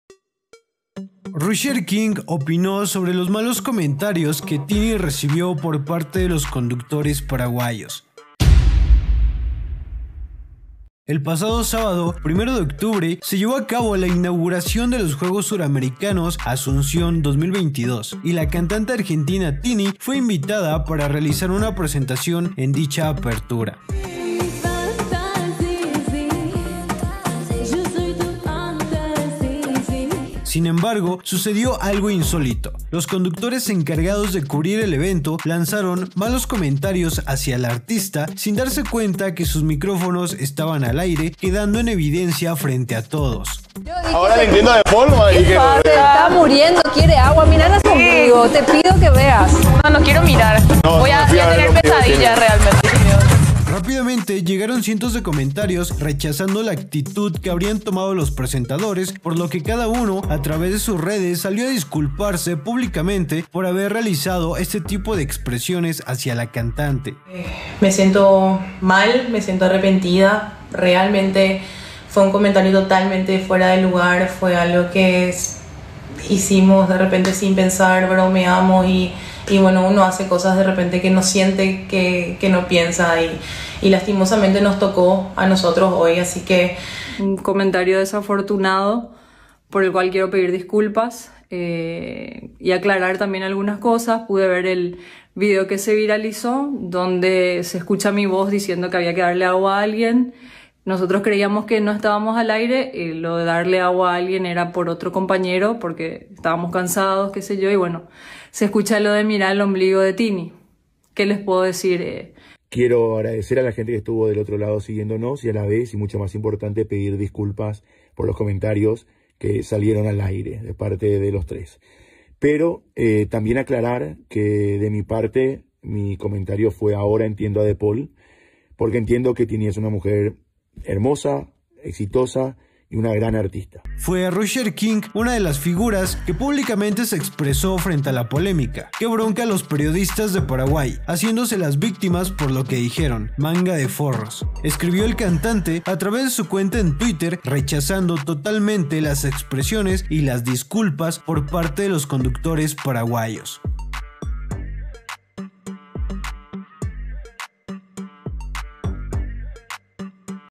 RUSHERKING opinó sobre los comentarios a TINI "Que bronca los periodistas haciéndose las victimas"
Rusherking opino sobre los malos comentarios que Tini recibió por parte de los conductores paraguayos.

El pasado sábado primero de octubre, se llevó a cabo la inauguración de los juegos suramericanos Asunción 2022, y la cantante argentina Tini, fue invitada para realizar una presentación en dicha apertura.

Sin embargo, sucedió algo insólito, los conductores encargados de cubrir el evento lanzaron malos comentarios hacia la artista, sin darse cuenta de que sus micrófonos estaban al aire quedando en evidencia frente a todos.

Rápidamente llegaron cientos de comentarios rechazando la actitud que habrían tomado

Los presentadores por lo que cada uno a través de sus redes salió a disculparse públicamente por haber realizado este tipo de expresiones hacia la cantante.

Fue Rusherking una de las figuras que públicamente se expresó frente a la polémica: "Que bronca los periodistas de Paraguay, haciéndose las victimas por lo que dijeron, manga de forros" escribió el cantante a través de su cuenta en Twitter, rechazando totalmente las expresiones y las disculpas por parte de los conductores paraguayos.